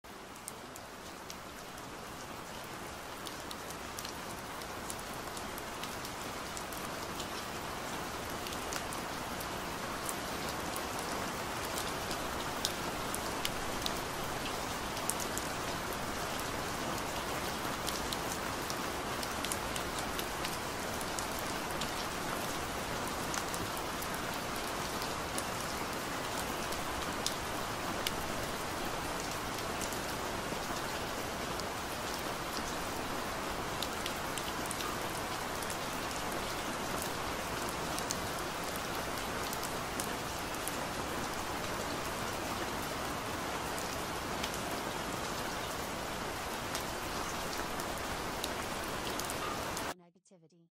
Student Council subliminalヽ(≧∀≦)ﾉ
Affs(^·^)

Be a student council member
Everyone respects and loves you
Have a great reputation
Be known for student council
Everyone votes for you
Have good grades
Be respected by everyone in school